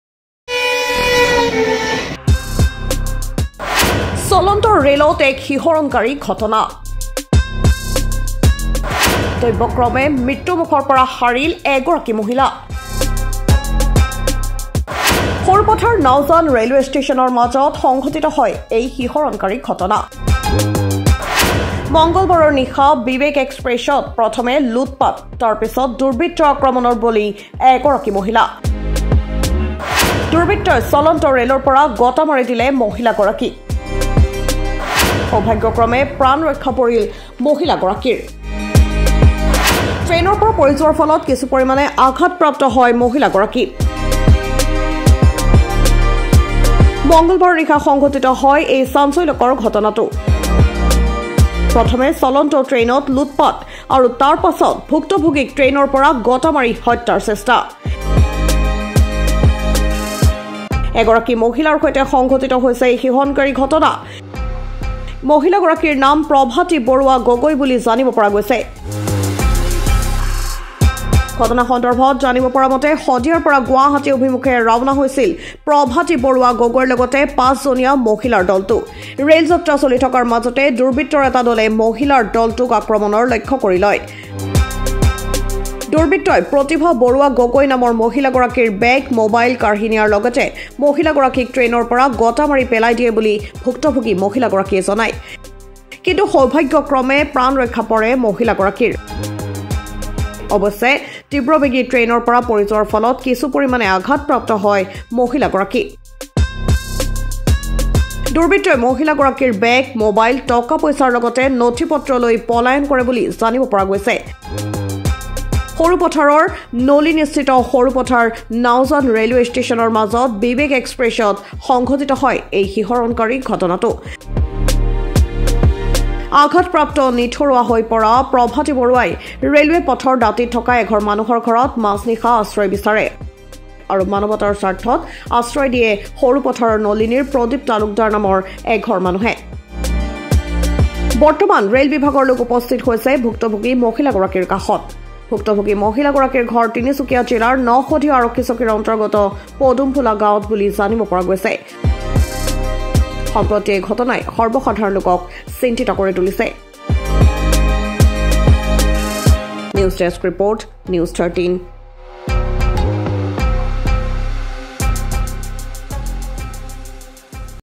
চলন্ত ৰে’লত শিহৰণকাৰী ঘটনা !
'দৈৱক্ৰমে মৃত্যুুুমুখৰ পৰা সাৰিল এগৰাকী মহিলা'
'সৰুপথাৰ-নাওজান ৰে’লৱে ষ্টেচনৰ মাজত সংঘটিত হয় এই শিহৰণকাৰী ঘটনা'
File Photo